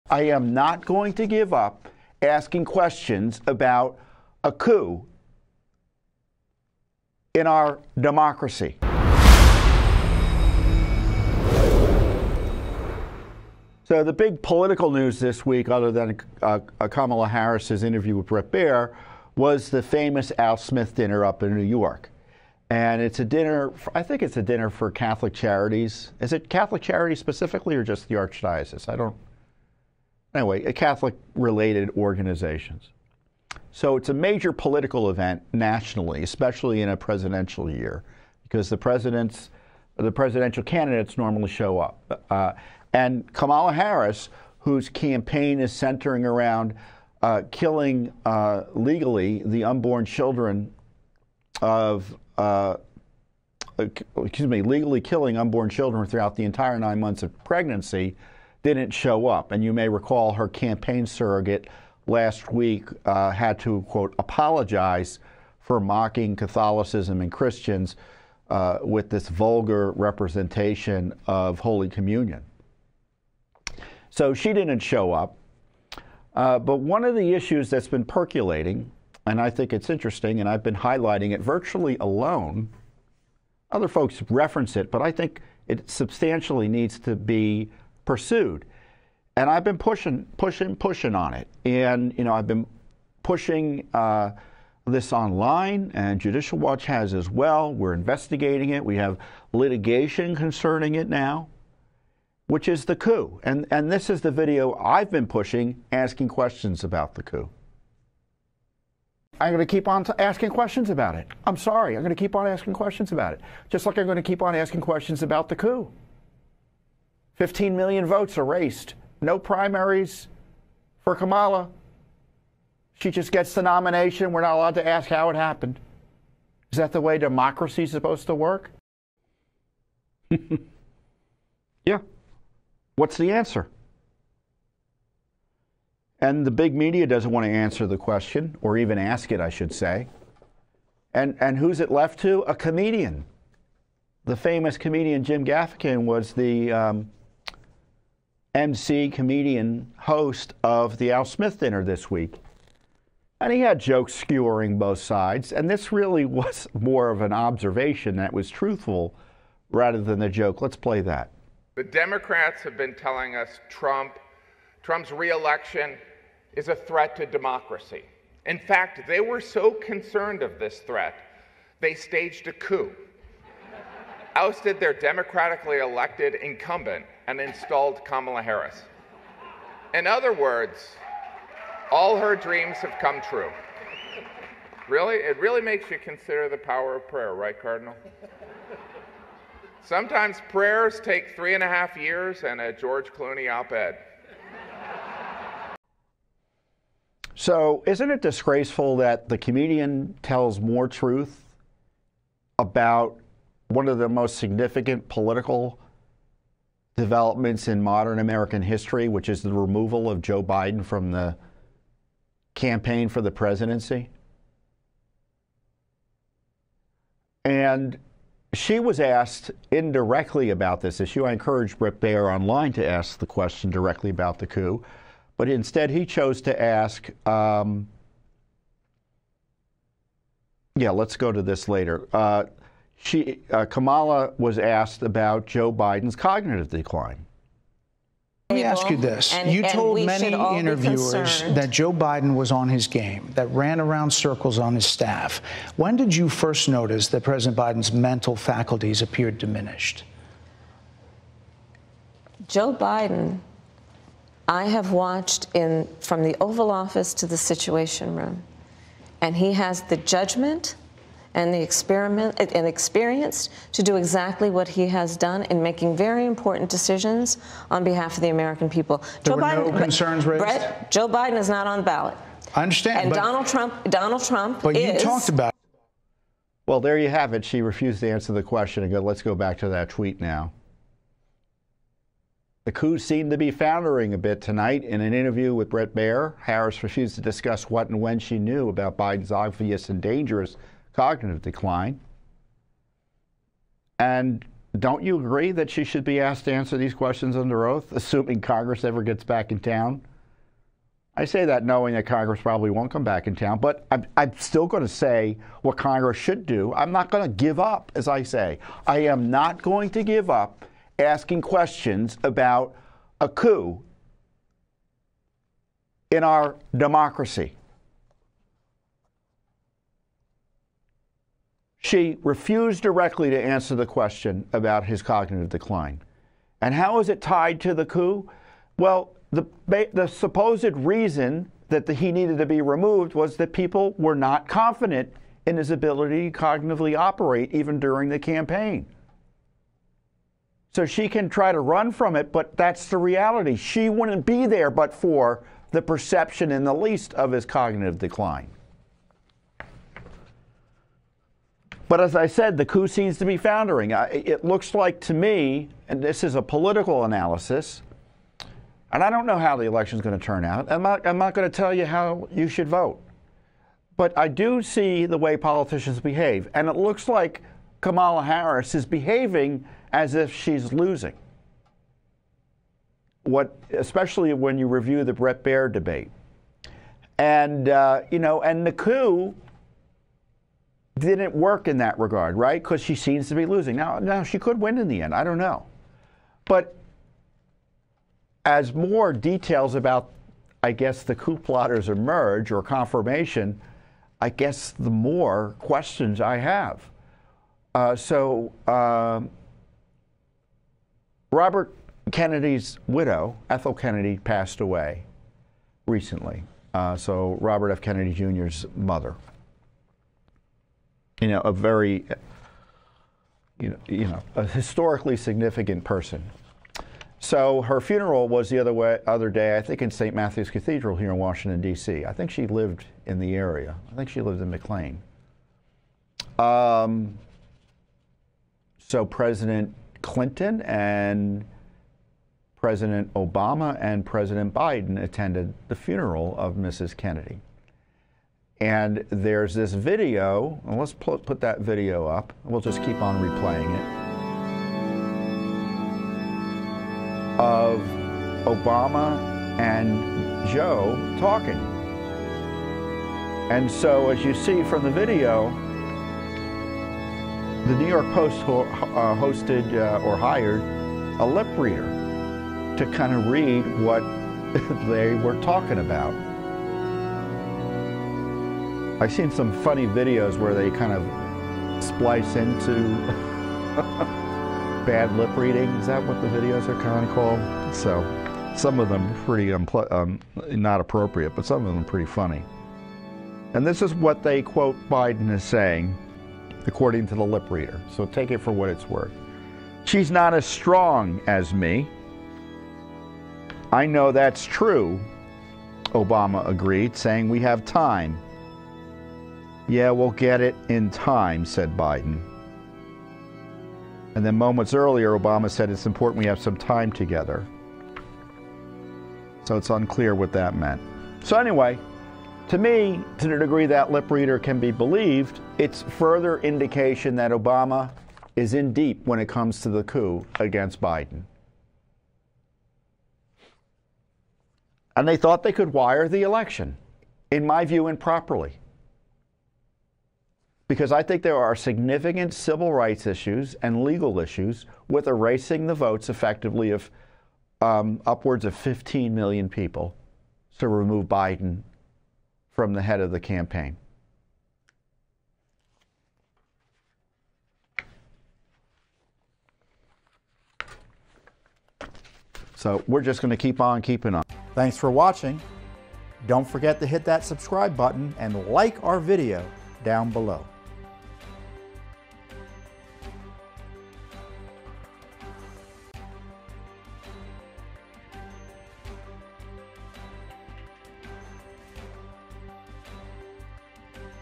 FITTON: “I Won’t Back Down Against the Coup!”
Judicial Watch President @TomFitton gives the latest update on the coup, how Kamala Harris presumably became the Democratic nominee and the ongoing crisis we face with Joe Biden's cognitive challenges.  WATCH NOW!